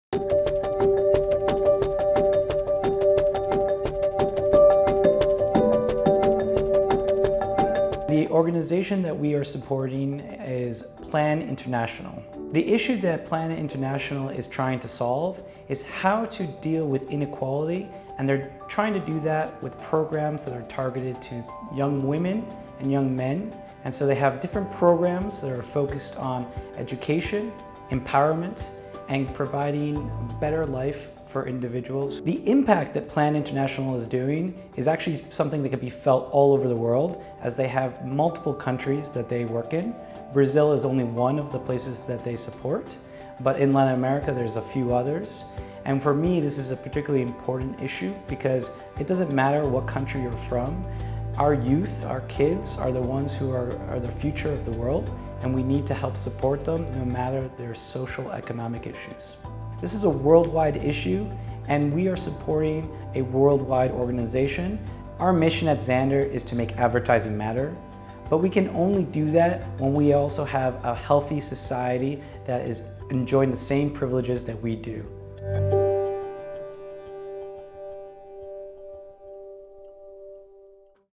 Xandr Cares 100K Challenge: Plan International
Plan International is a humanitarian organization that advances children’s rights and equality for girls. Plan International has been nominated by Patrick Casal, Senior Director, LatAm Market Development, for the Xandr Cares $100k Challenge.